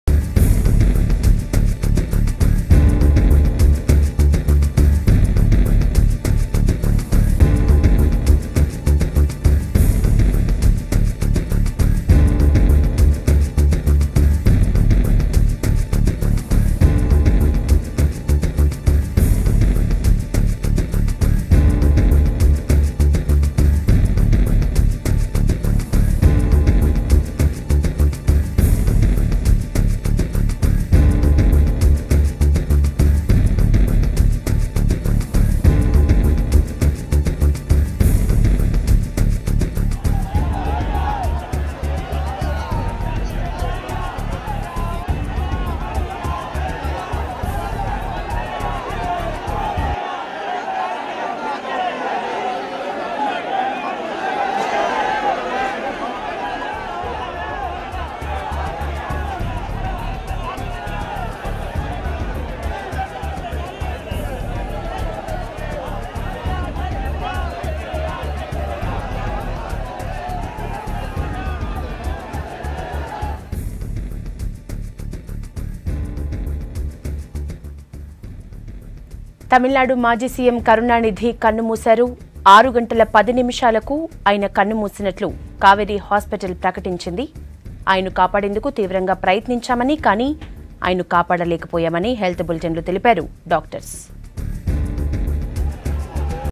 DMK Chief Karunanidhi Passes Away In Kauvery Hospital | V6 News
DMK Chief Karunanidhi Passes Away at the age of 94 In Chennai's Kauvery Hospital.